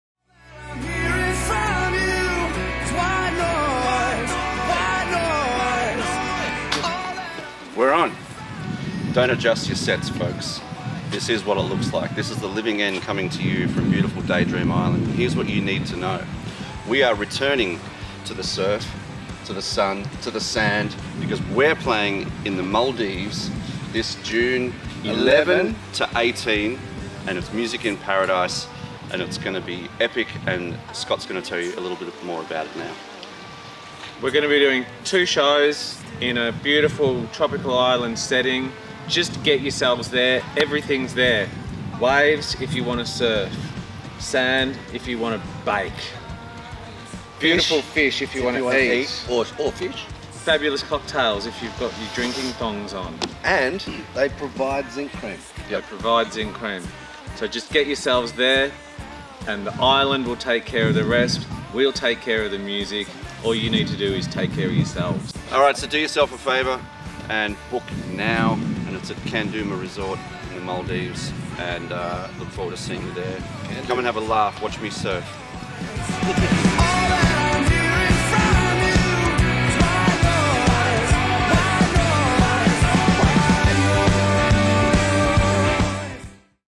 The Living End invite you to the Maldives in 2023
“The ultimate and incredibly intimate gigs in paradise”
Join The Living End, Tex Perkins and Matt Walker for 4 incredibly personal gigs on this specially curated Music in Paradise event in the heart of the warm, sunny, tropical Maldives at beautiful Kandooma Resort.
This is a music experience and week-long holiday in paradise not to be missed! The Living End will perform two very special, intimate and extended live performances. The charismatic combo of Tex Perkins and Matt Walker are also performing two up-close, personal shows.
Wow… what a lineup!